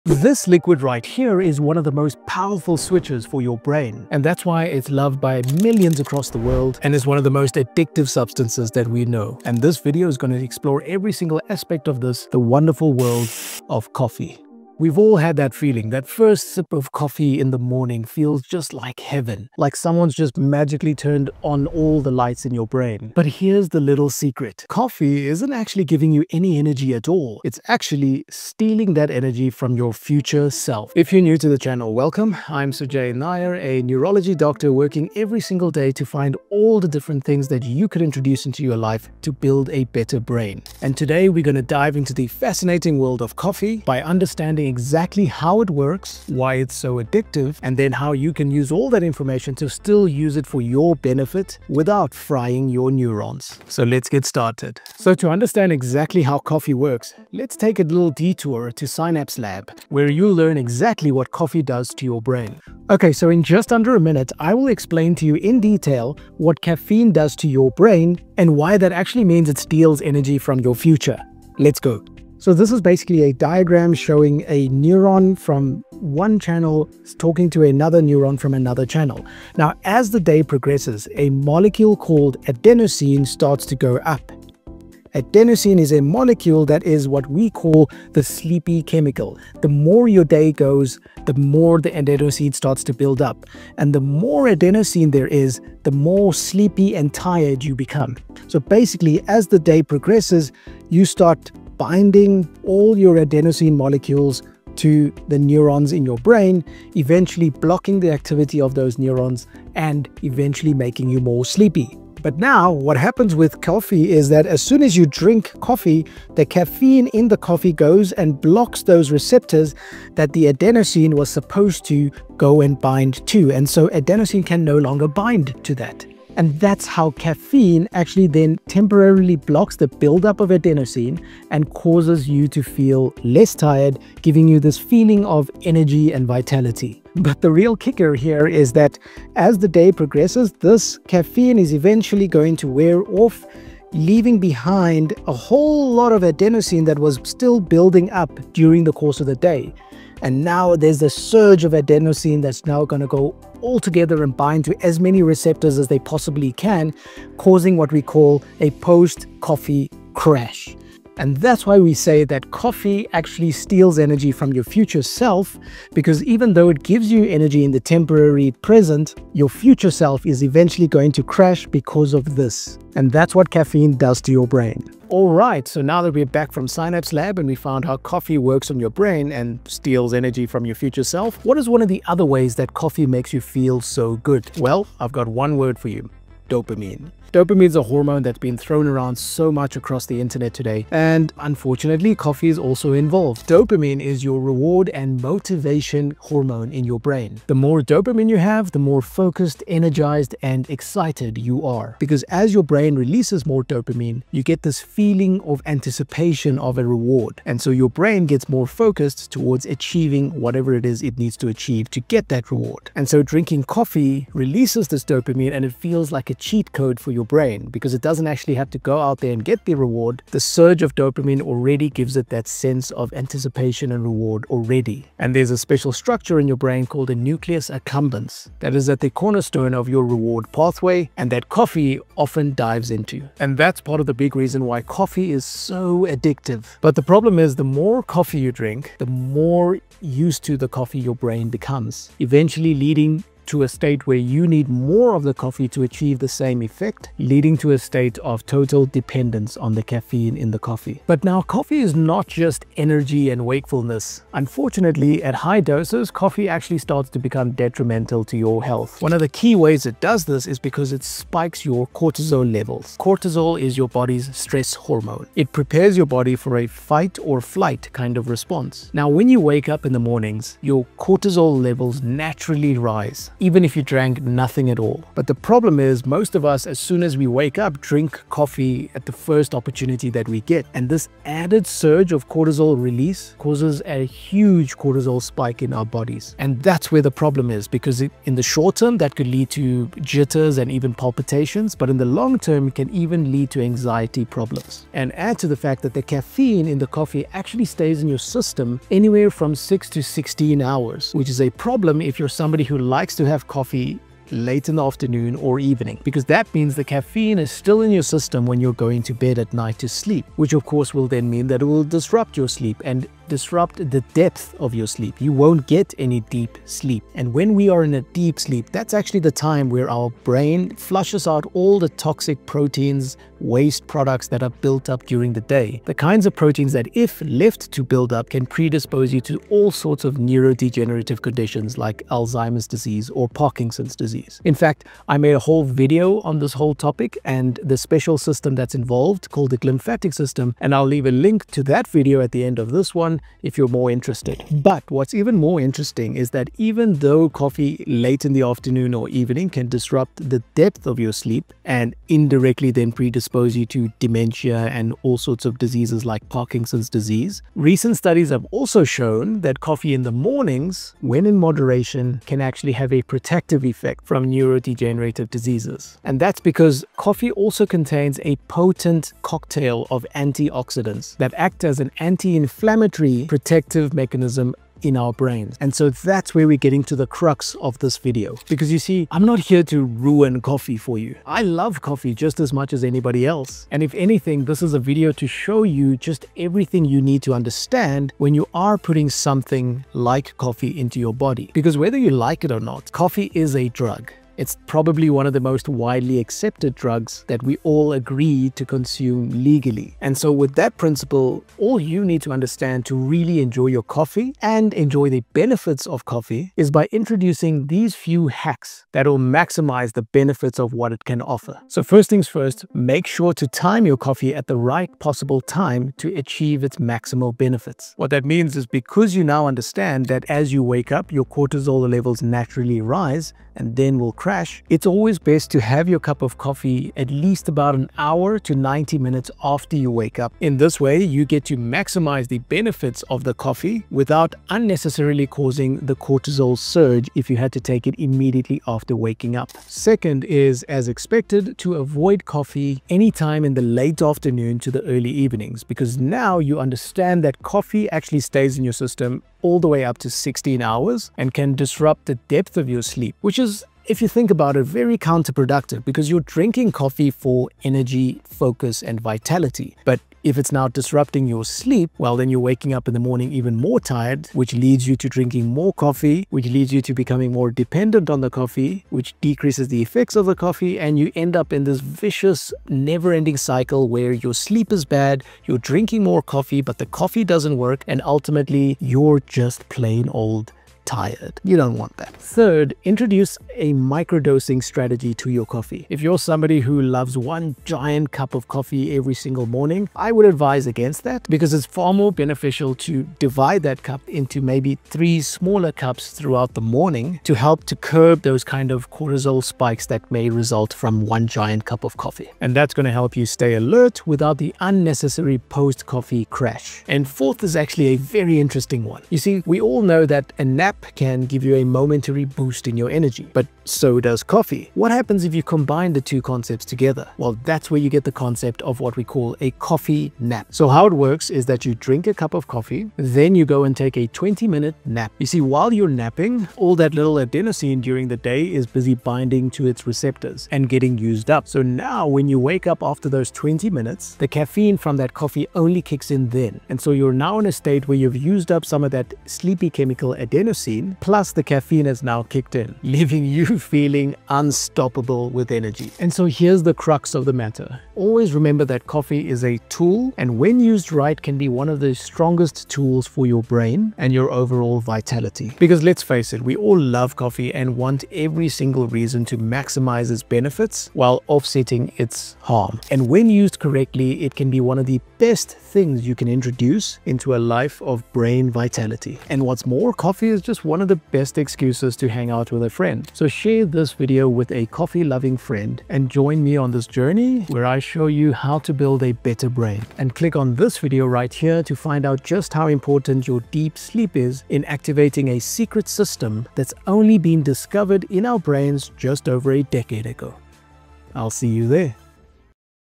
The Surprising Truth about Coffee and Your Brain, Revealed
What if your daily coffee fix is secretly borrowing energy from your future self? Discover the hidden impact of caffeine on your brain, including the benefits of coffee, the dangers of coffee and how you can use coffee to build a better brain.